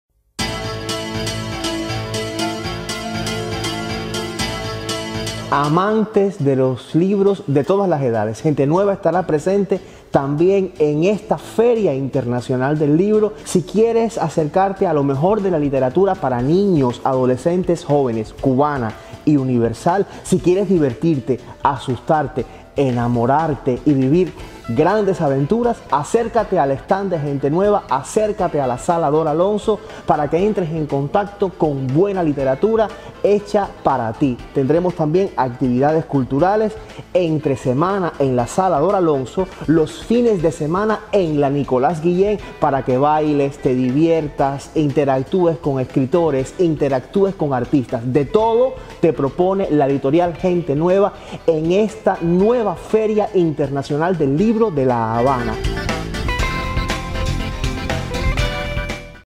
FILH2023 - Maikel Rodríguez Calviño (escritor)
El evento literario más grande del país, que tendrá como país invitado de honor a Colombia, contará con la participación de reconocidos escritores colombianos y un amplio programa profesional.
📚 La fiesta cultural para toda la familia cubana contará con la presencia de más de 40 países y de 120 expositores nacionales y foráneos. Asimismo se contará con más de 4200 títulos y más de 250 novedades de libros en papel.
📚 Será una oportunidad donde confluirán literatura, arte y cultura... Del 9 al 19 de febrero...